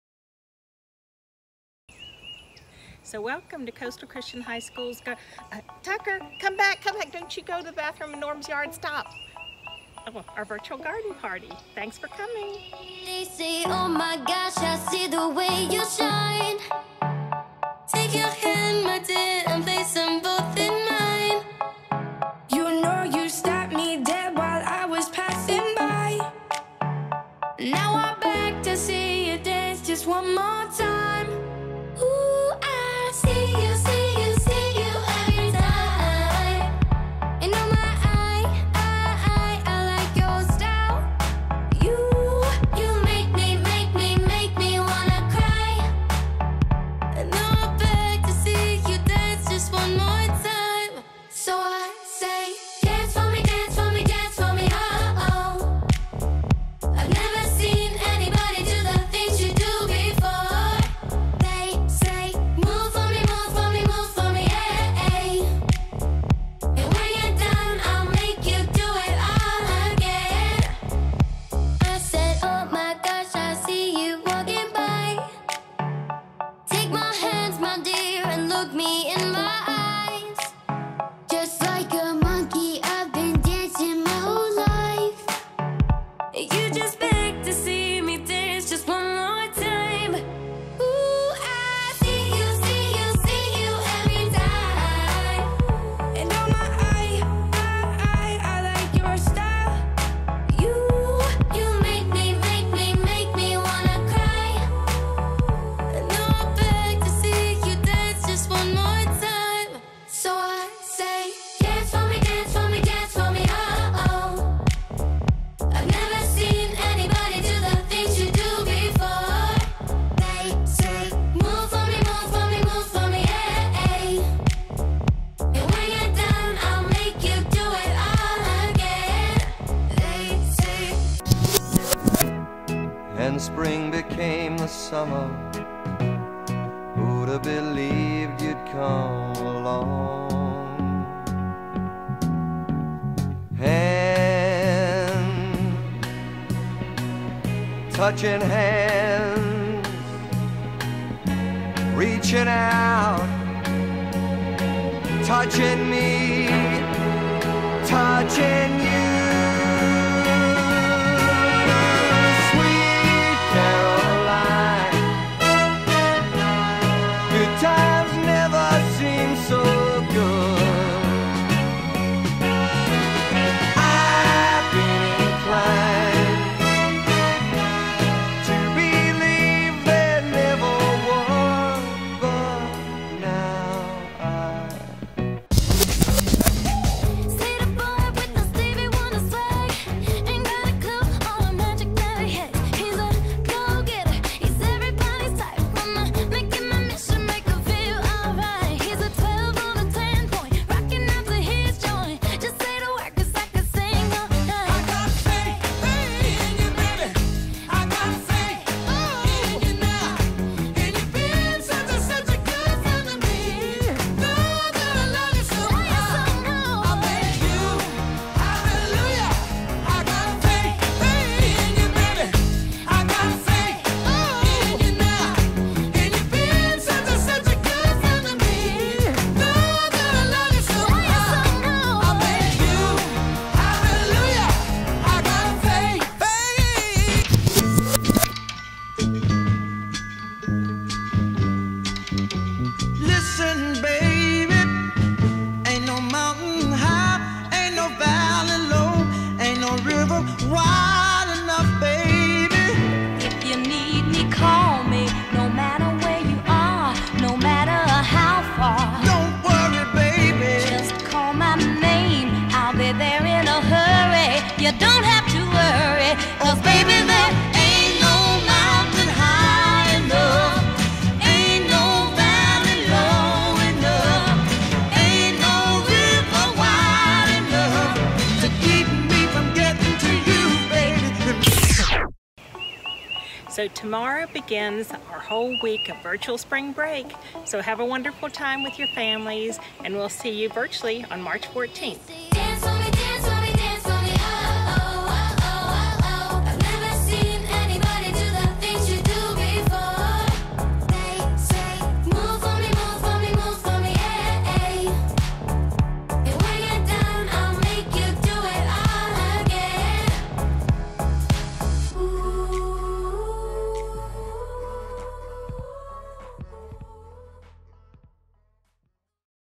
CCHS Virtual Garden Party
Students and Teachers contributed to this virtual garden party with a little help from TicTok...